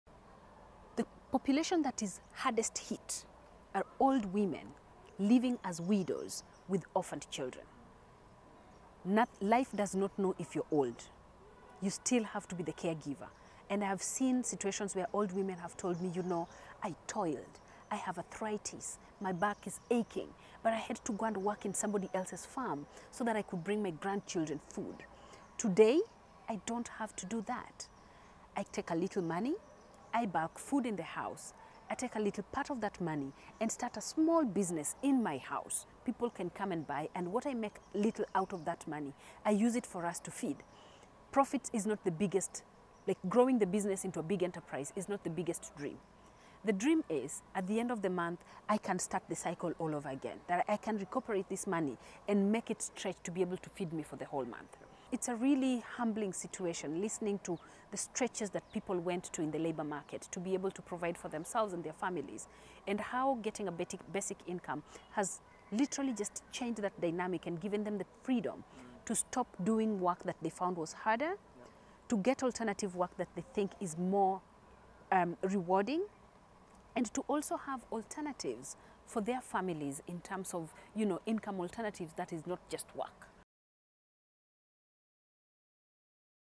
Basic income is life transforming for older women
Caroline Teti from the Kenyan NGO Give Directly speaks on the power of basic income at the 19th Basic Income Earth Congress, held in Hyderabad in August 2019.